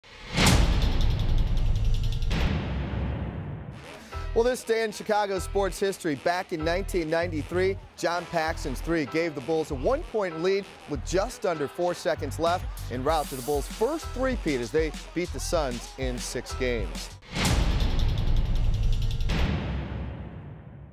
John Paxson made his clutch 3 in Phoenix 23 years ago today
Bulls guard John Paxson's clutch 3-pointer that put the Bulls ahead for good in the deciding game of the 1993 NBA Finals in Phoenix happened 23 years ago today.